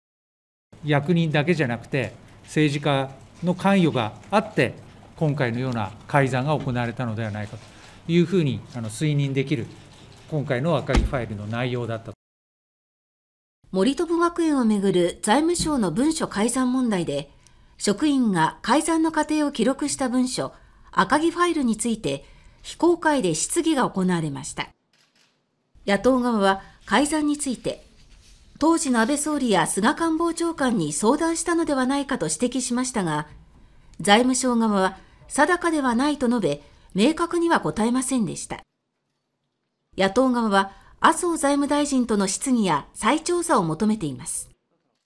赤木ファイルで質疑 「政治家の関与あったと推認」(2021年6月24日)
「赤木ファイル」が国会でも開示されました。

　立憲民主党・階衆院議員：「役人だけじゃなくて政治家の関与があって今回のような改ざんが行われたのではないかというふうに推認できる今回の『赤木ファイル』の内容だった」

　森友学園を巡る財務省の文書改ざん問題で職員が改ざんの過程を記録した文書「赤木ファイル」について非公開で質疑が行われました。

　野党側は改ざんについて「当時の安倍総理大臣や菅官房長官に相談したのではないか」と指摘しましたが、財務省側は「定かではない」と述べ、明確には答えませんでした。

　野党側は麻生財務大臣との質疑や再調査を求めています。